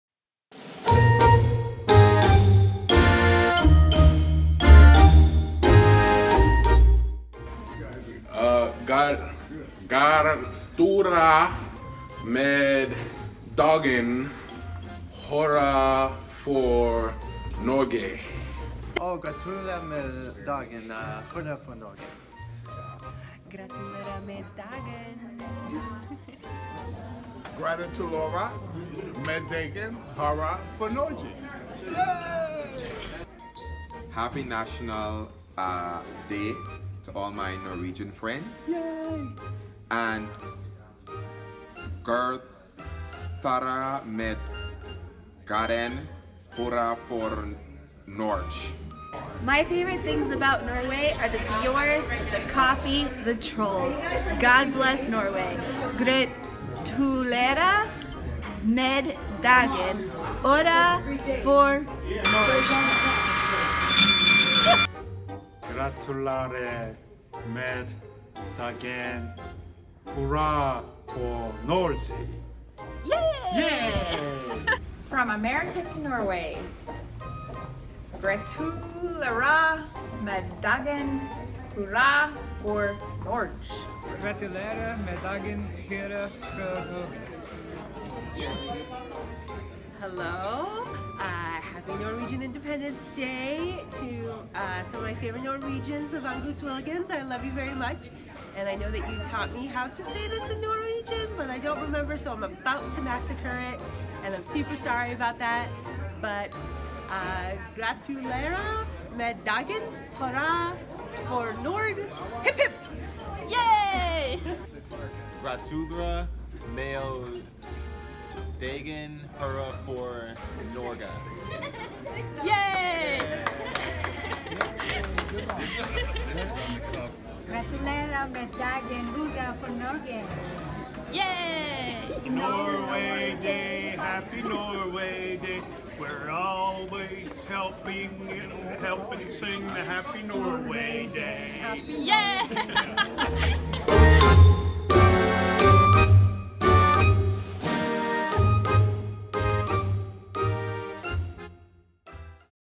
"Gratulerer med Dagen" from America...with Love!
Americans wish Norway "Gratulerer med Dagen" to celebrate the 17 of May. or at least they try to!